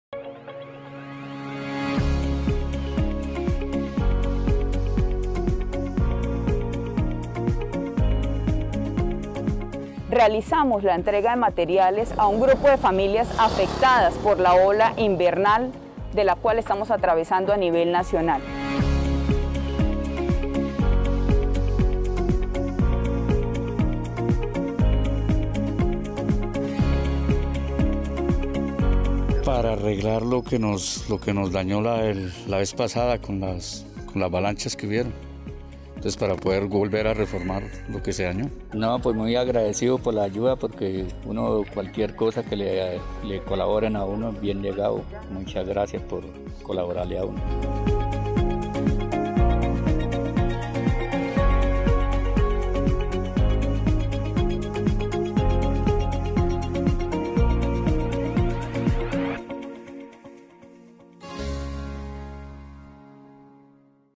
FLORIDABLANCA Materiales a familias de la vereda Rosa Blanca.
EN FLORIDABLANCA- La oficina de Gestión Ambiental y Mitigación del Riesgo realizó la entrega de material de construcción familias de la vereda Rosa Blanca.